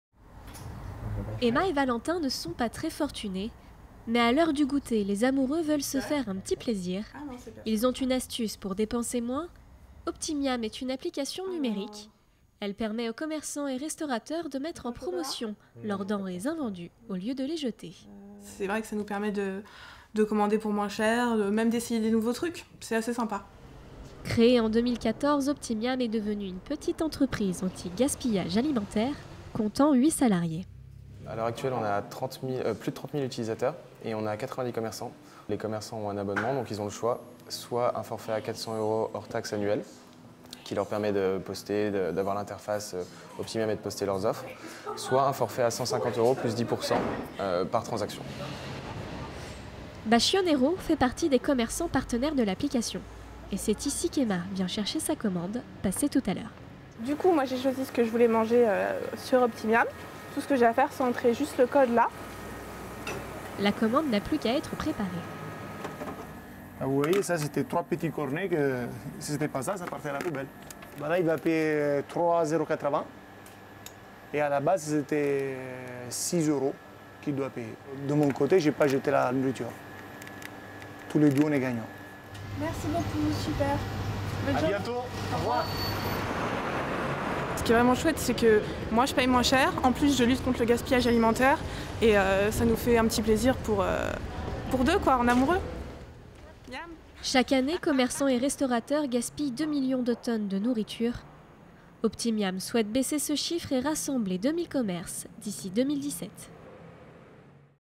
Optimiam : une application pour économiser et moins gâcher !
A l’heure où le gaspillage alimentaire est une préoccupation majeure, des jeunes entrepreneurs ont eu une idée pour lutter contre.